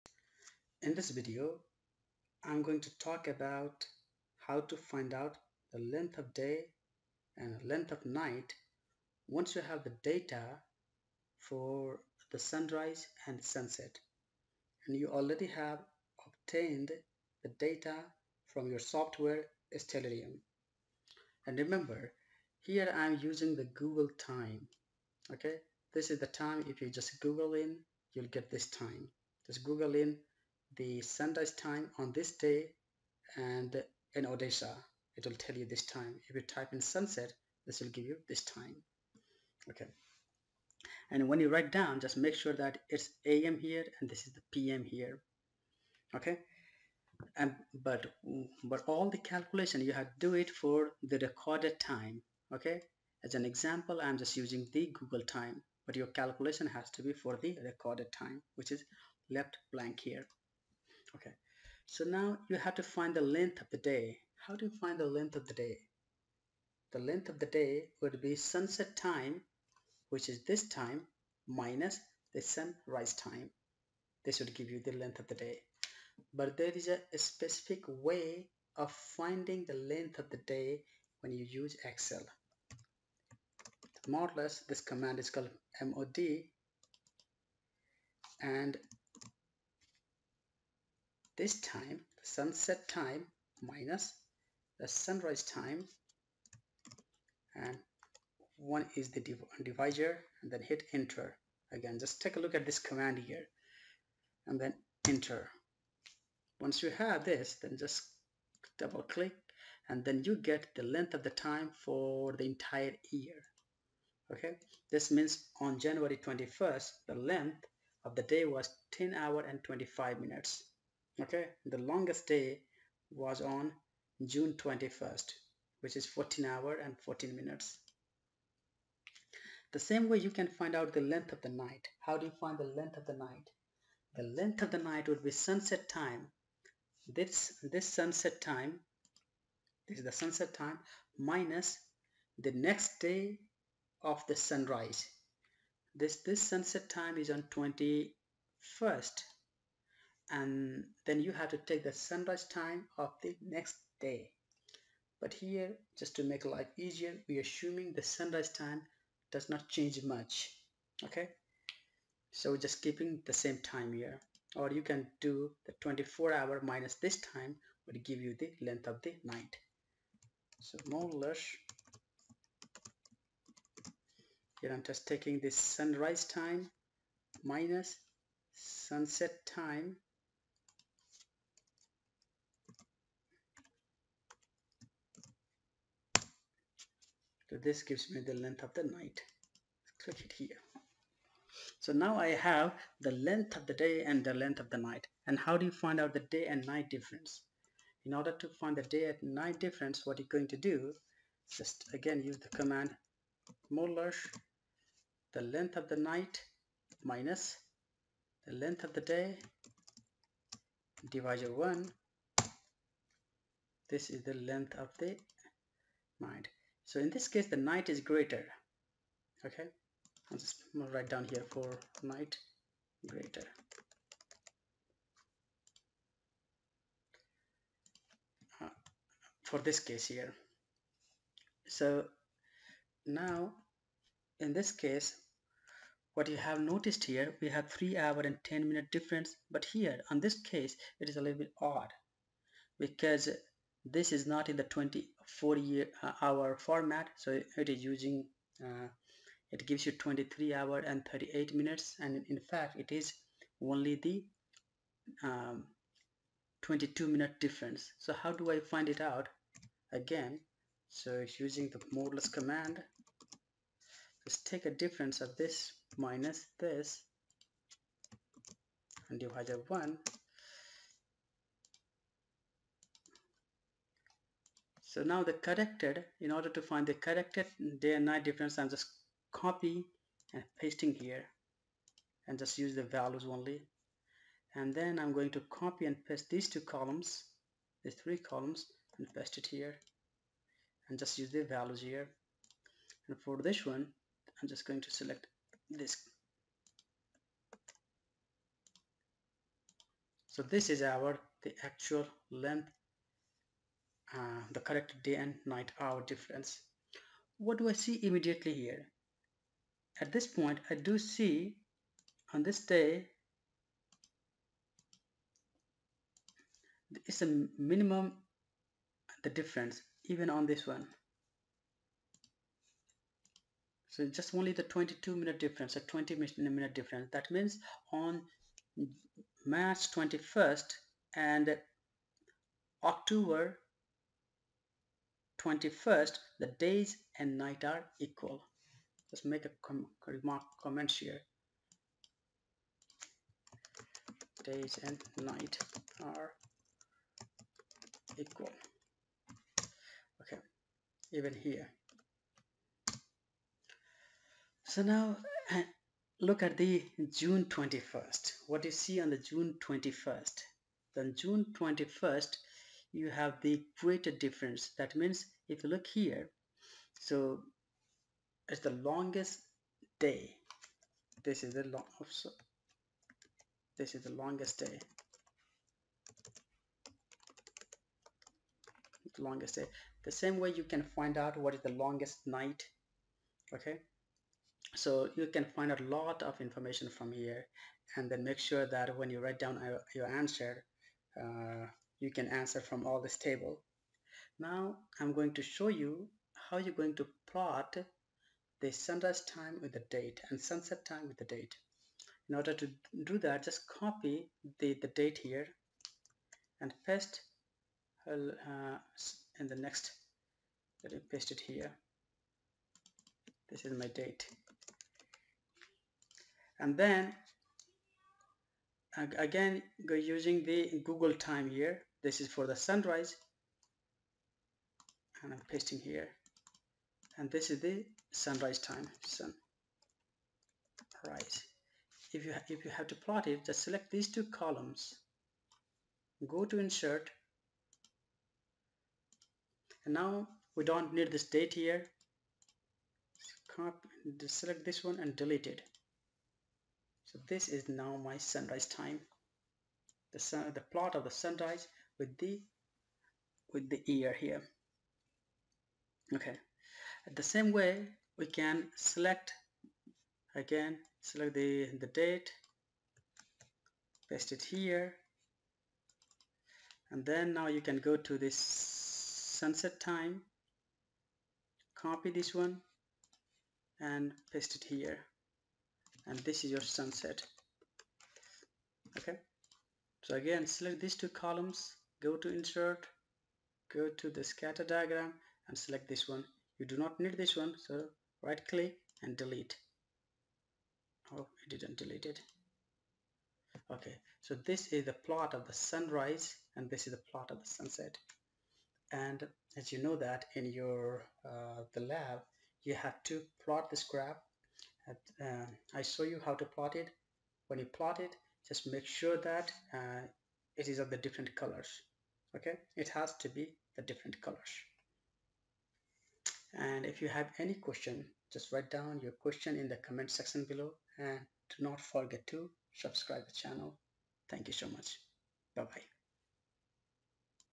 How to Find the Length of Day and Night # Using Excel
a) How to use Excel to calculate the time difference.
b) How to plot the graph in Excel